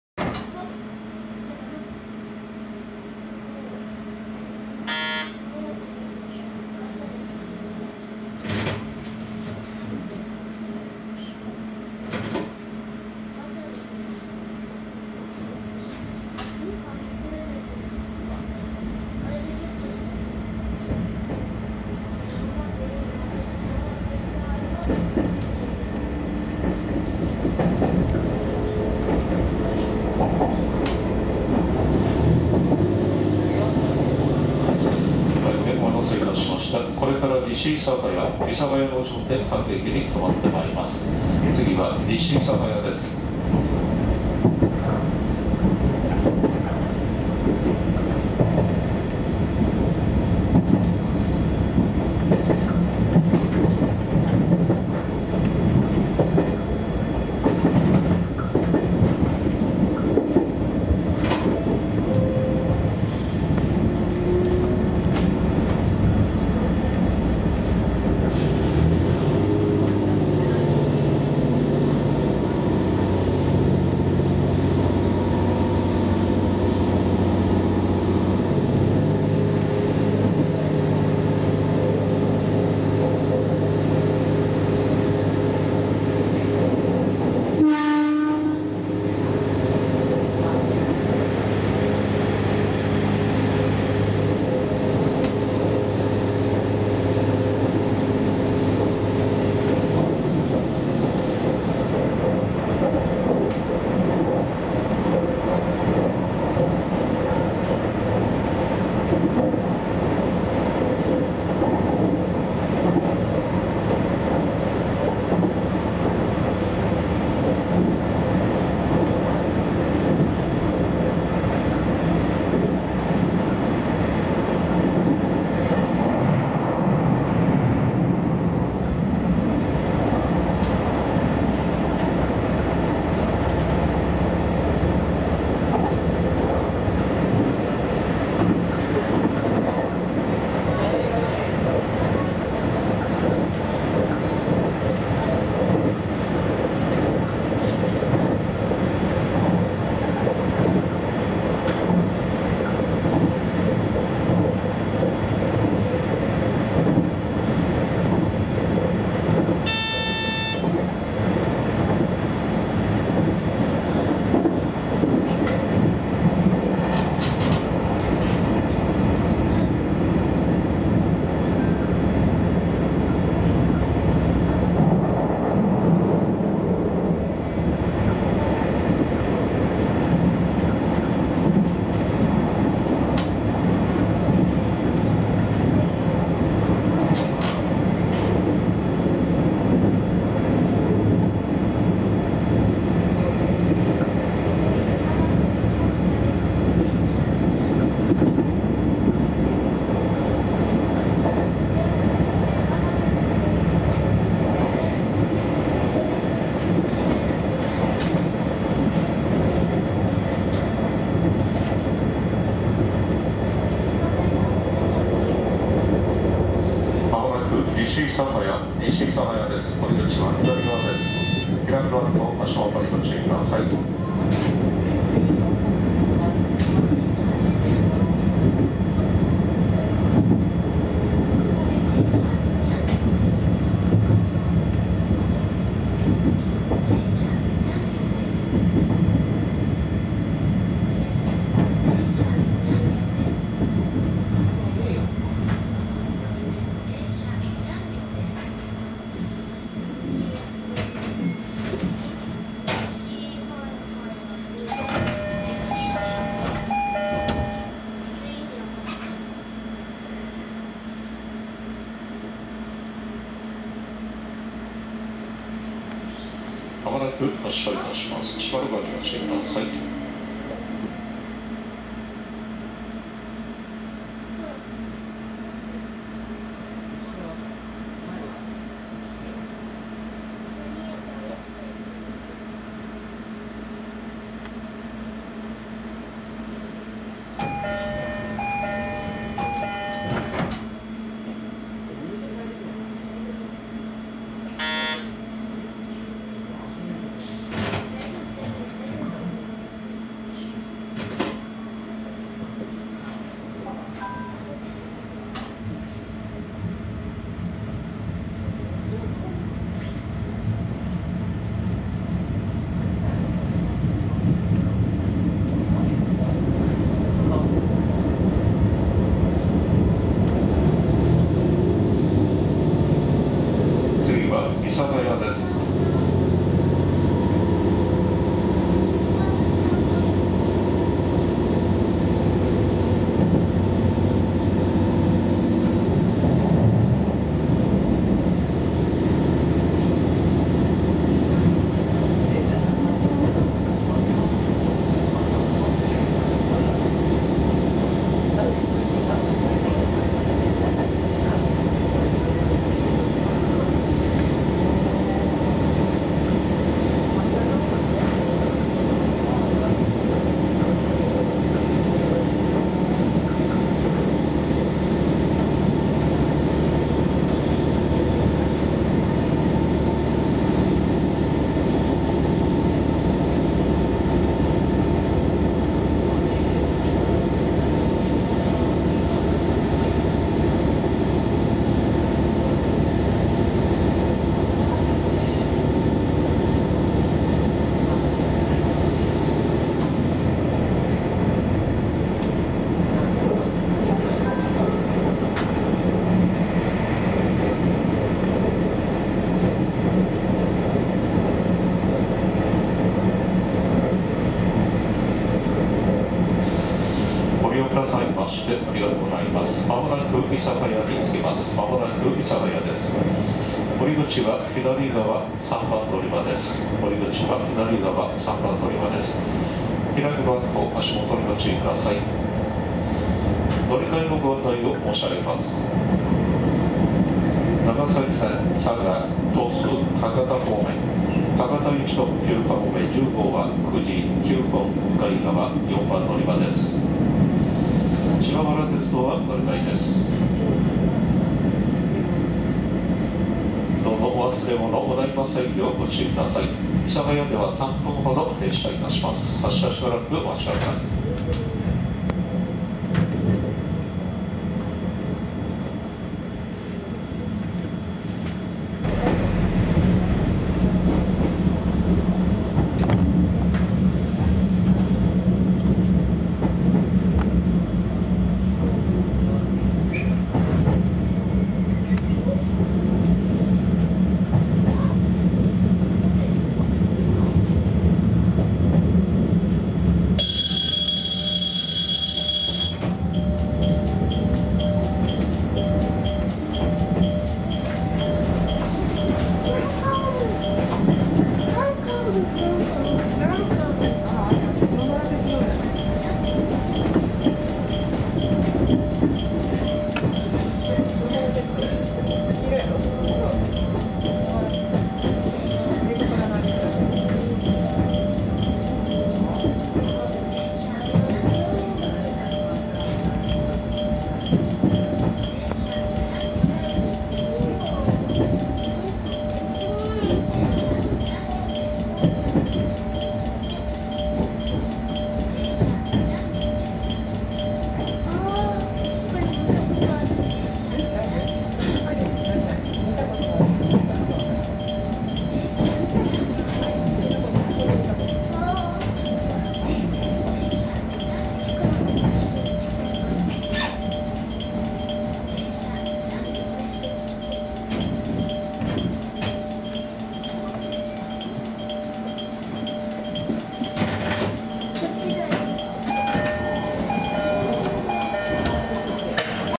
【YC1系前面車窓】喜々津→西諌早→諫早【長崎本線】
JR九州長崎本線（電化区間）YC1系より撮影した喜々津駅→西諌早駅→諫早駅の前面車窓です。喜々津～諫早駅は長崎本線複線電化区間です。

【撮影機材】
Panasonic 4K VZX990M 64GB HC-VZX990M-W
4K ビデオカメラ VZX992M

Everyday Update!